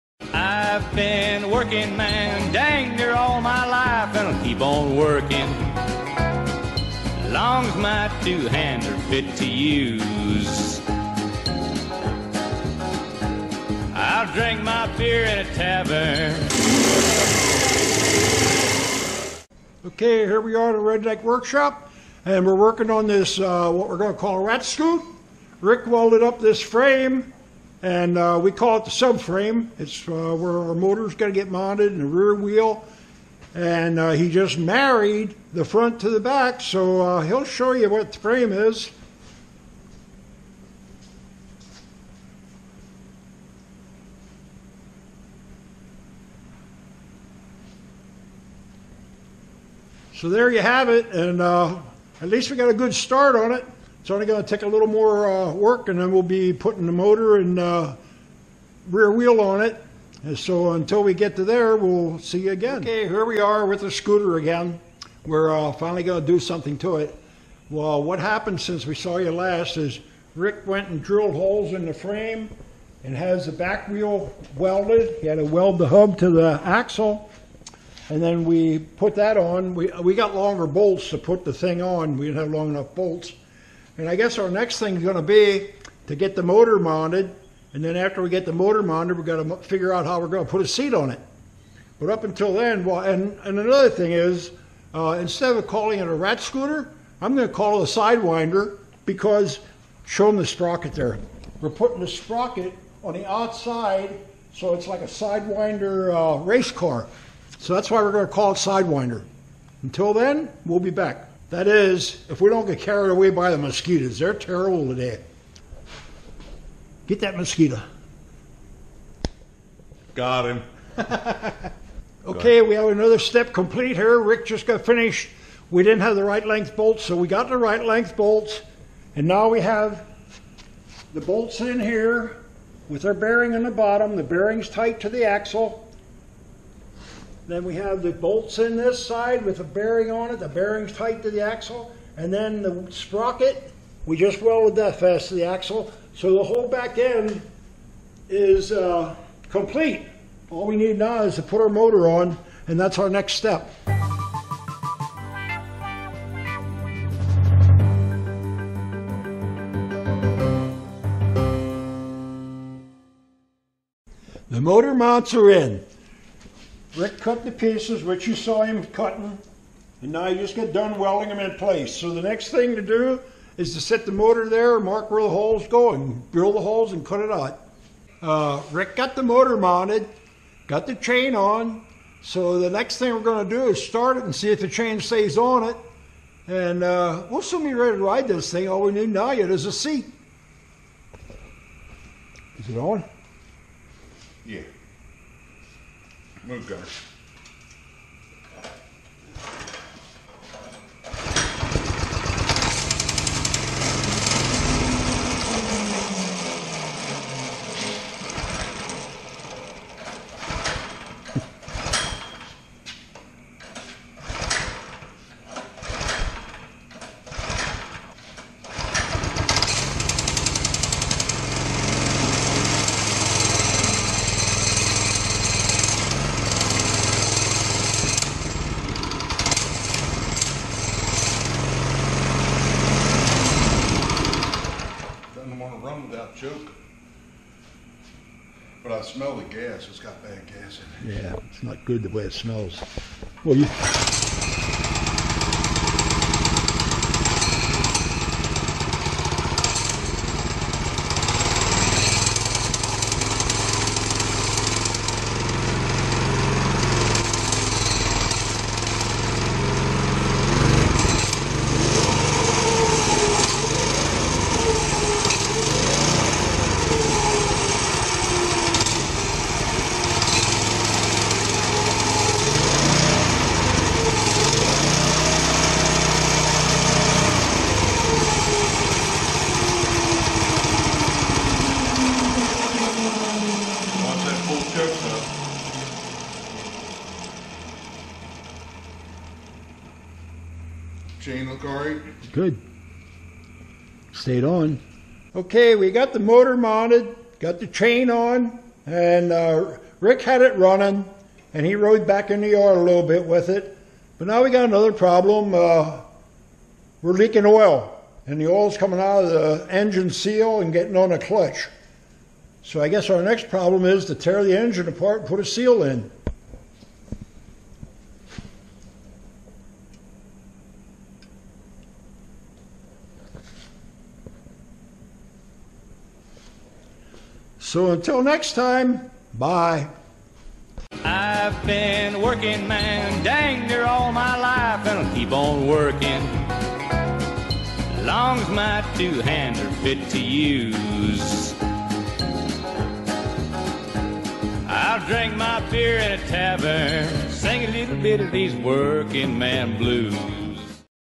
Pop Pop Gringos - project Series - Rat Scooter Part 2
Home made Scooter made from Scrap steel and broken Scooter...
And a few headaches along the way...